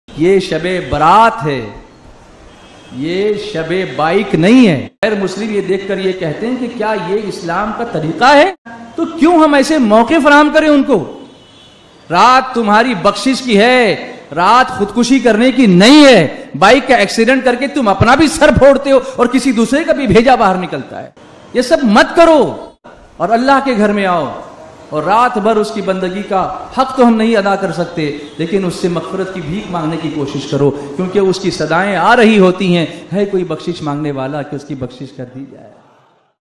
Ye Shabe Barat Hai Shabe Bike Nahi | Qari Rizwan Sahab Bayan | Nawjawano Ko Takid | Ibadat Ki Raat
*Watch Our New Videos On*👇👇👇👇
*HERA ISLAMIC CHANNEL JALGAON*👇

Hera Islamic Channel Jalgaon Is The Best Platform To Get